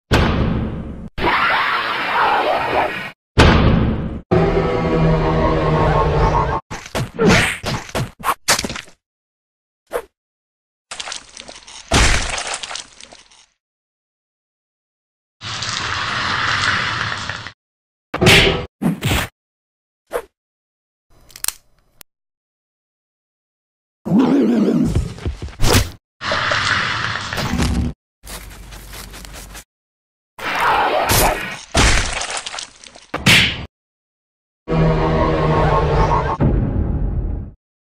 SNMFC: Kaiyos vs Bugo
This is my original alien fighting an alien hunter, I might make a Kaiyos story if you guys want?

Originally animated on March 25, 2024